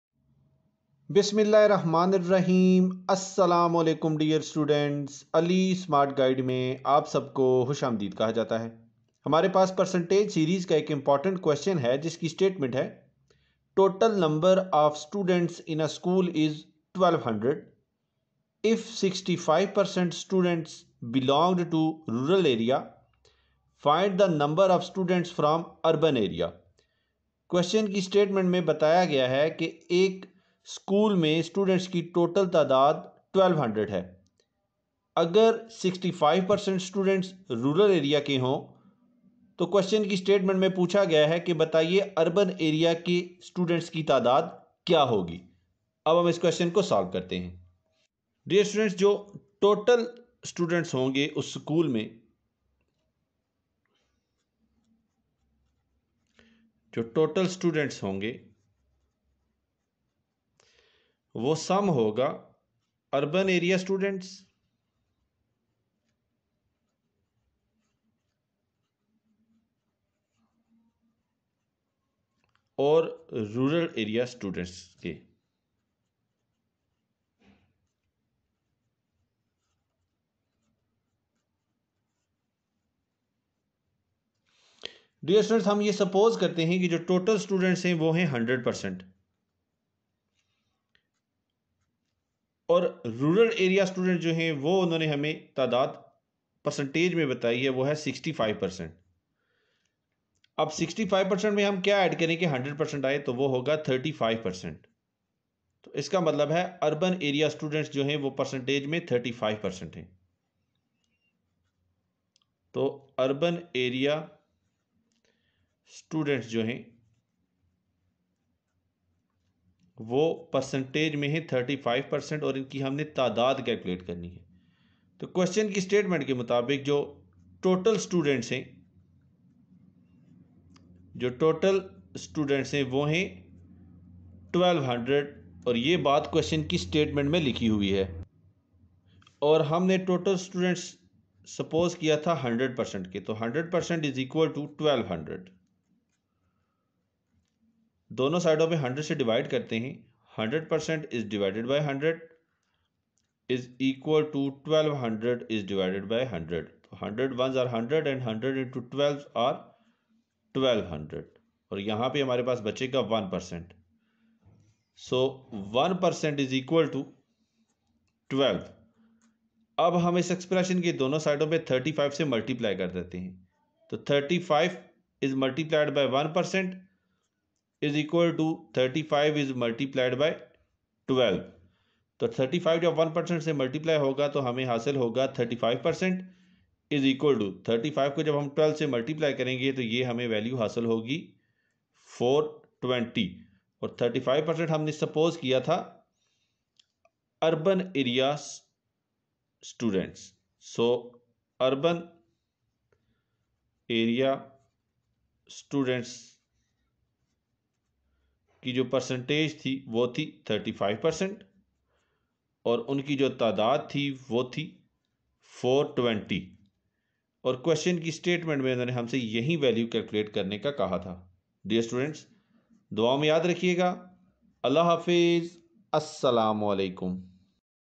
Total number of students in a School is 1200 if 65% students belonged to rural area find .....
Shortcuttipsandtricks discussed in this video lecture will enable the candidates for the preparation of
basic math mcqs, basic maths for entry test, basic mathematics for competitive exams, basic mathematics for ppsc,